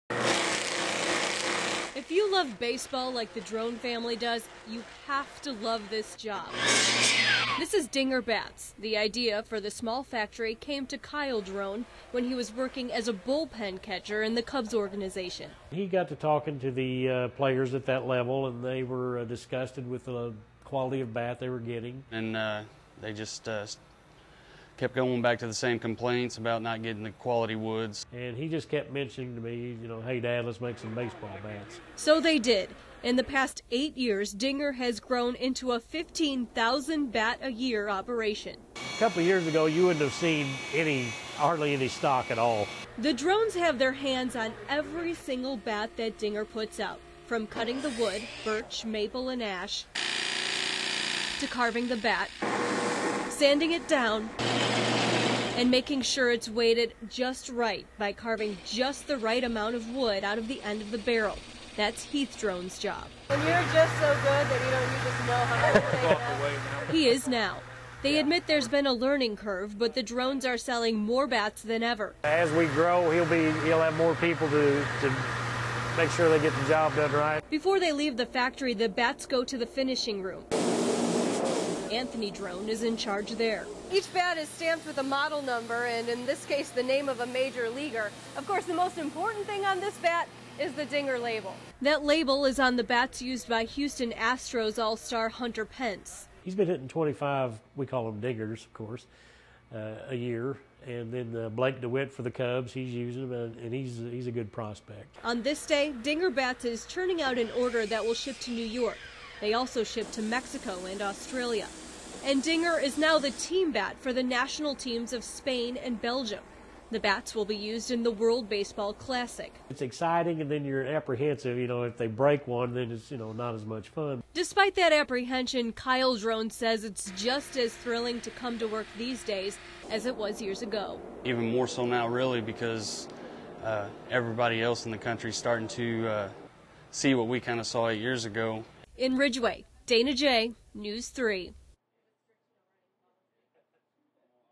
Dinger Bats
A look at how a local family is making their bats for players all over the world.